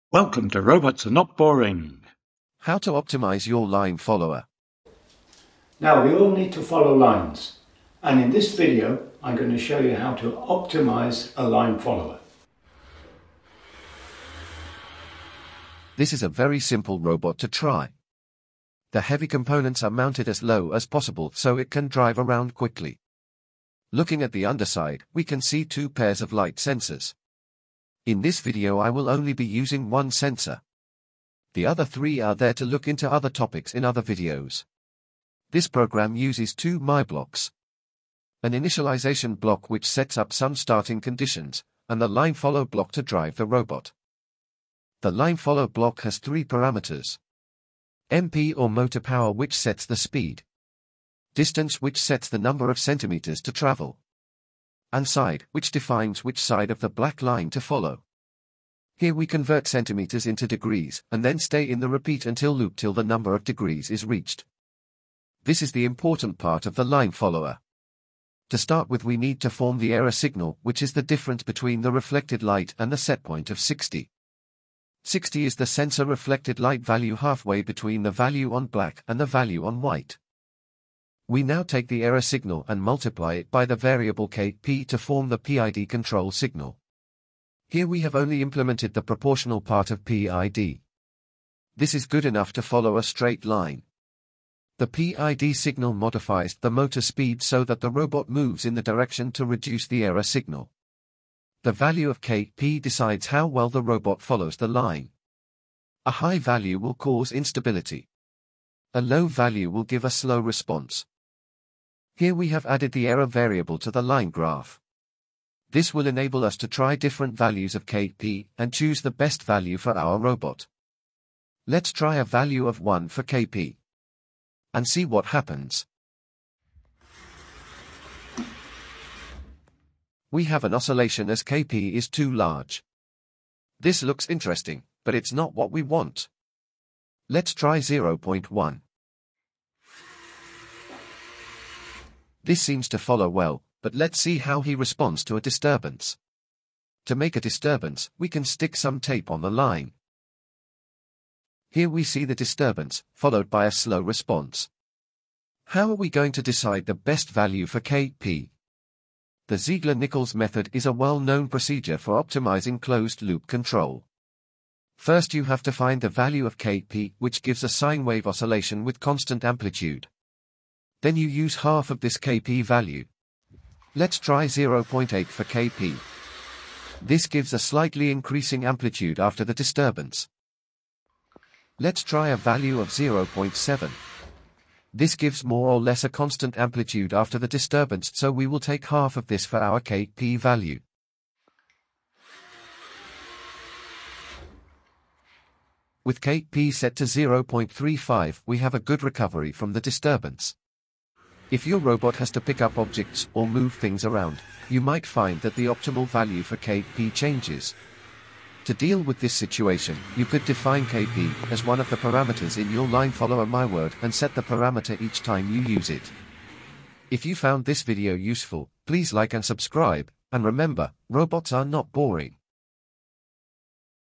Mastering Linefollower Optimization with Spike Prime!
This video explains how to optimize a proportional line follower using a Spike Prime robot. A well tried but simple method is shown for finding the best value for Kp, the proportional gain factor.